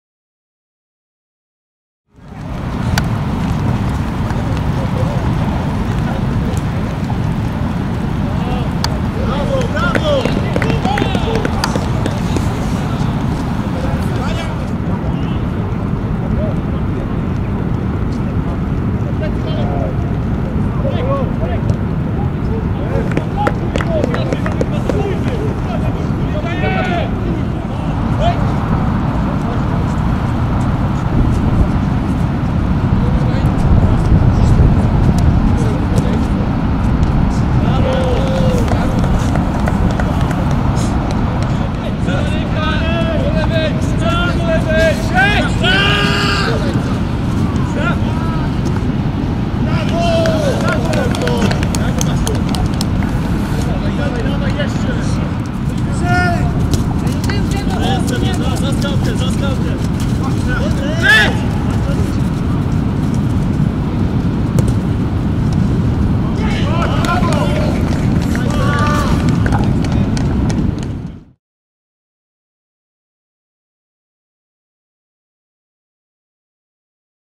WIDEO: sparing Stal Rzeszów - Wólczanka Wólka Pełkińska [BRAMKI]
Stal Rzeszów pokonała czwartoligową Wólczankę Wólka Pełkińska 5-0 w meczu sparingowym. Zobaczcie gole z tego spotkania.

STAL RZESZÓW - WÓLCZANKA WÓLKA PEŁKIŃSKA 5-0 (2-0)
1-0 Arkadiusz Gil
2-0 Oleg Kvych
3-0 Arkadiusz Gil
4-0 Maciej Maślany
5-0 Michał Szymański